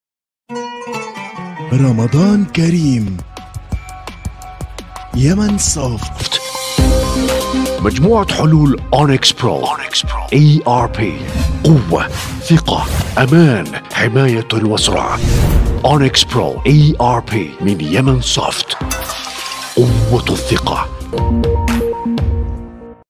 حلول إدارة شحن الطرود اونكس_ برو من
نظام شحن الطرود:
 هو نظام مخصص لاستخدام منشآت شحن الطرود دولياً أو محلياً وفقاً لسياسة تسعير حسب الأوزان والأقاليم.

من أهم وظائفه:
تهيئة الأسعار والخصومات حسب الإقليم والعملة وشرائح الوزن.
تحديد بوليصات الدفع المسبق وتهيئة تسويقها للعملاء.
إدخال بيانات الطرود وإصدار بوليصة الشحن سواءً كانت داخلية أو خارجية بأسعارها ومعلومات وجهتها ومستلمها.
إثبات بوليصات شحن التسعيرات الخاصة للعملاء سواءً كانت نقدية أو آجلة.

من أهم مميزاته:
مرونة تهيئة الأقاليم حسب ما تقرة المنشأة في سياستها للتسعير.
الاحتساب الآلي للأعباء المستحقة كالضريبة وغيرها.
إمكانية احتساب عمولات للمندوبين بحسب سياسة المنشأة.
رقابة البوليصات المستخدمة وغير المستخدمة ومصادرها واتجاهها.
الحصول على تقارير إجمالية وتفصيلية بالمبيعات والأعباء وغيرها.

يثبت نظام الشحن قدرة مجموعة أنظمة أونكس برو ERP على التكامل مع متطلبات عملاء الشحن في التأثير على الانظمة المالية والمحاسبية.